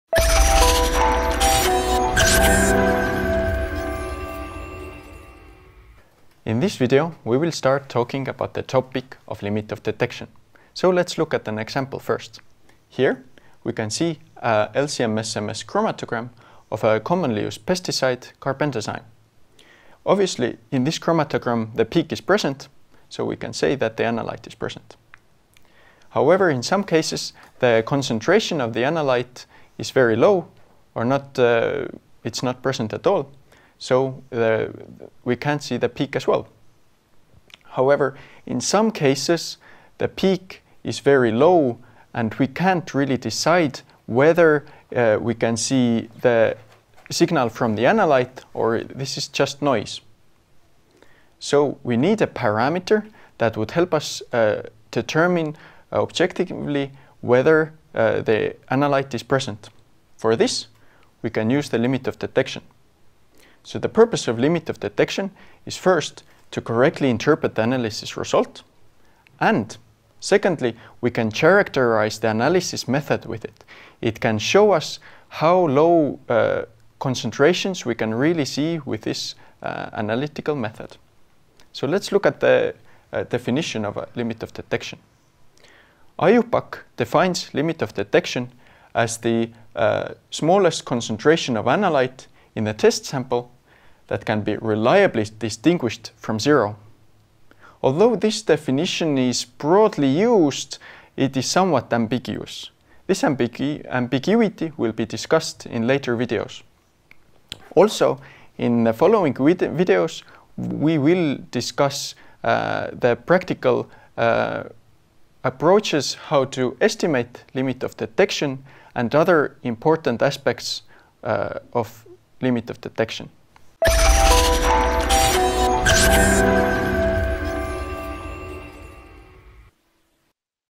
Limit of Detection
This video is from a free MOOC about LC-MS method validation which can be found in the following address